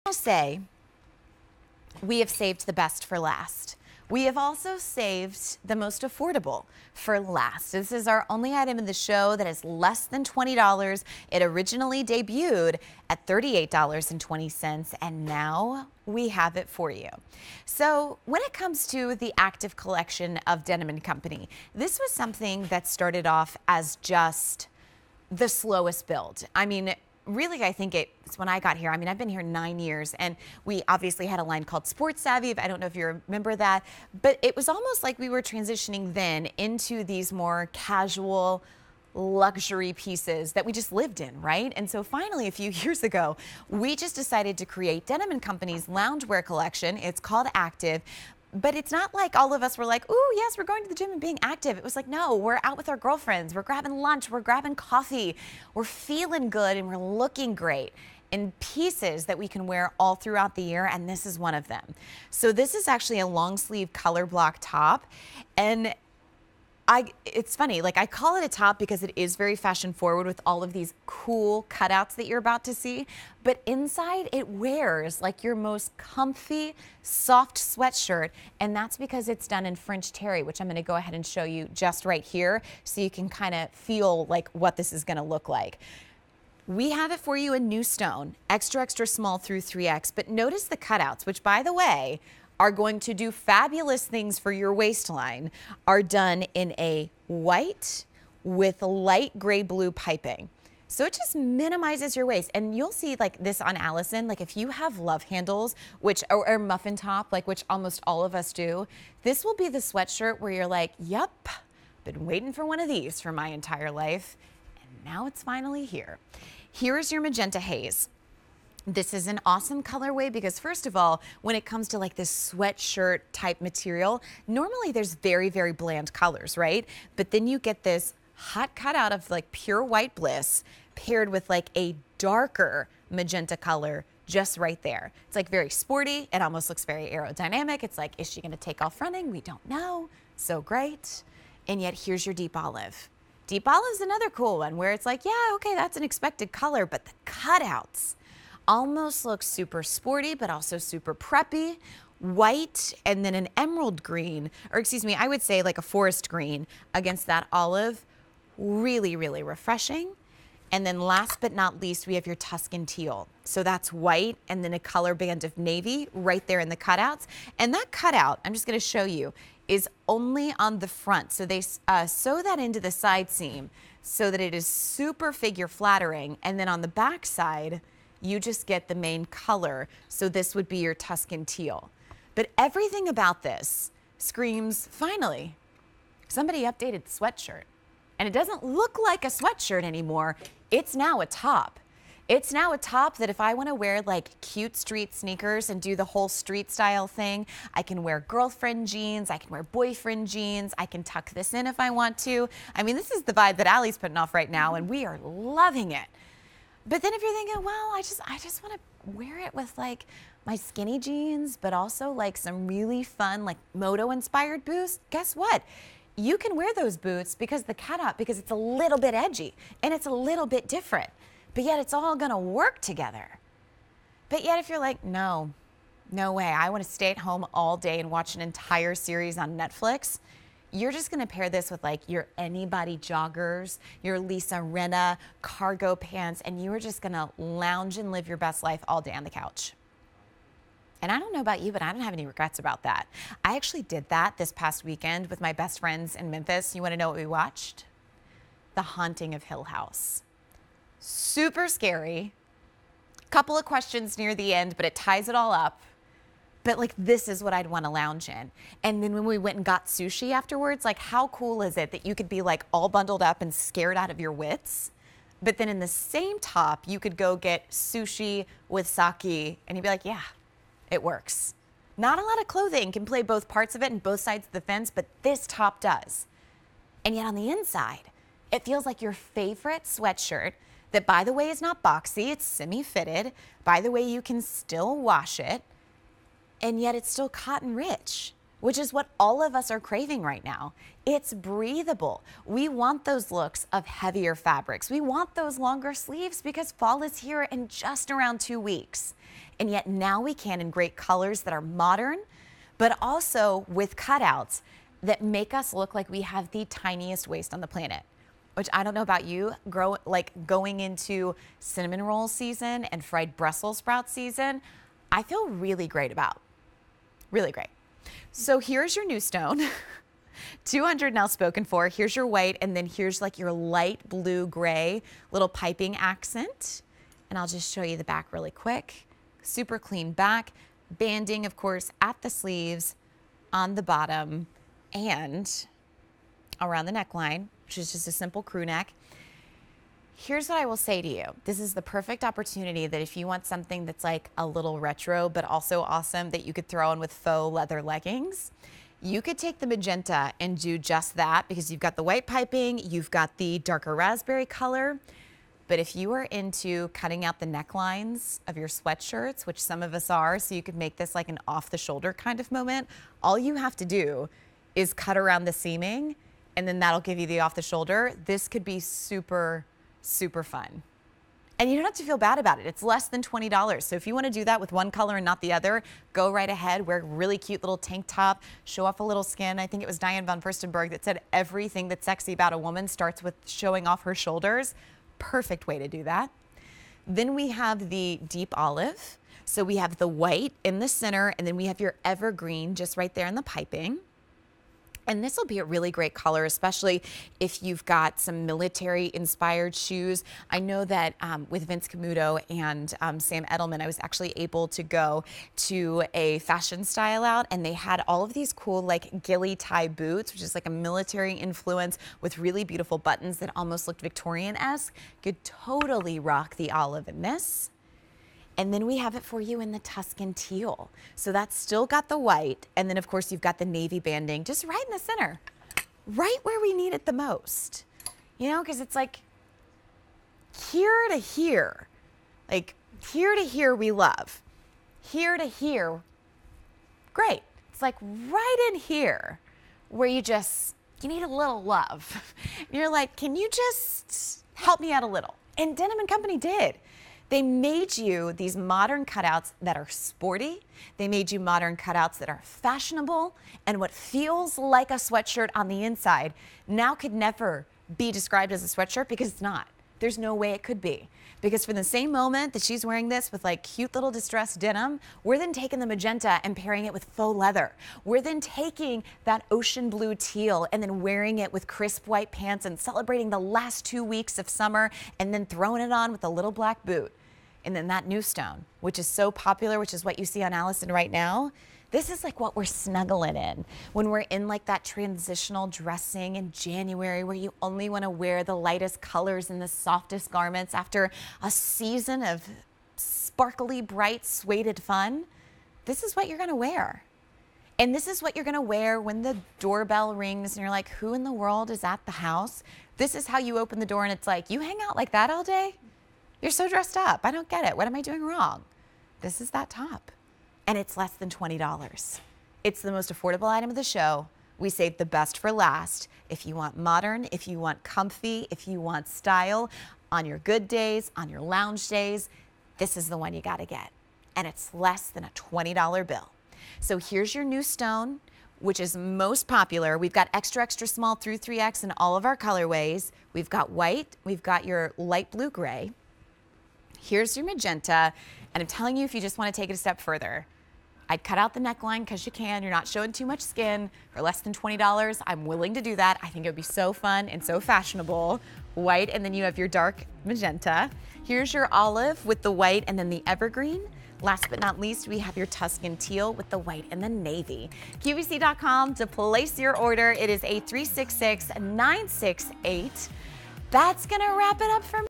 Denim & Co. Active French Terry Long-Sleeve Color-Block Top on QVC
Denim & Co. Active French Terry Long-Sleeve Color-Block Top

Cool morning jogs on the beach have nothing on this soft color blocked top. From Denim & Co.&reg; Fashions.